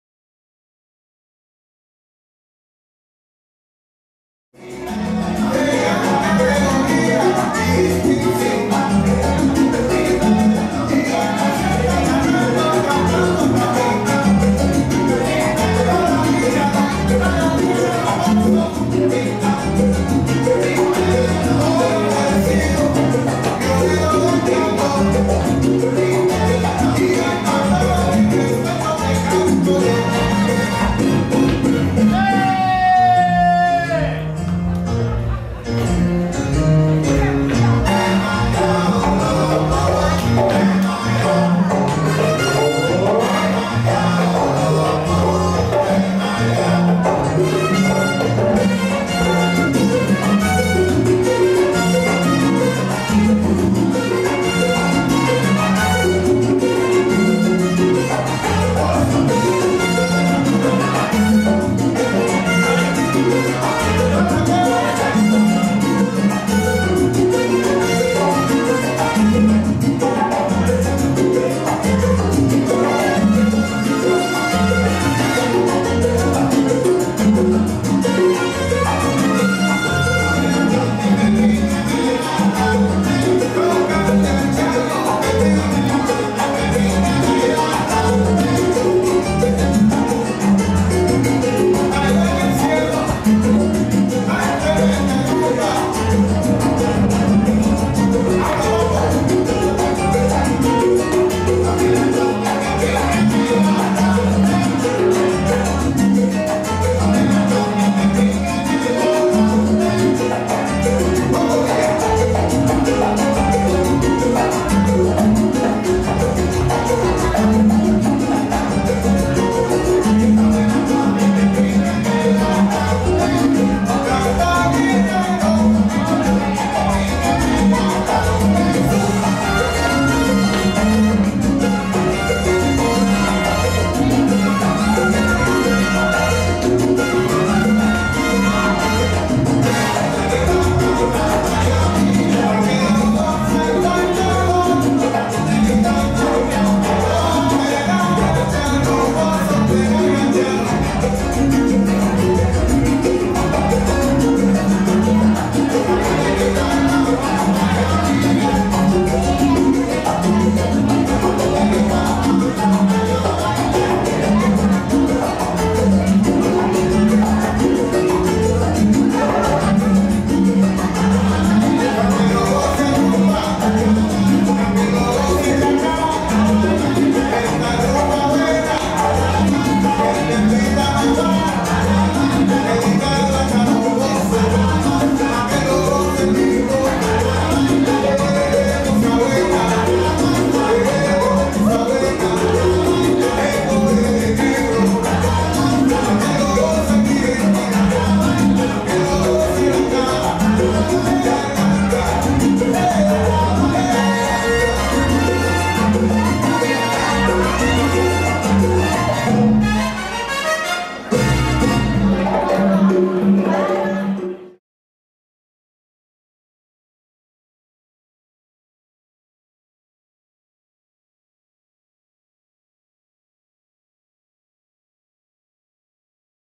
Adolfo Indacochea and Tatyana Rakhmetulova Salsa Dancing at Rostov For Fun Fest, Sat 02.11.2019 (SC)
Adolfo Indacochea and Tatyana Rakhmetulova Salsa Dancing at Rostov For Fun Fest (Russia), Saturday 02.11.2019 (SC)

Next festival: 30 apr-4 may 2021 - Respublika Days (10 years old dance festival).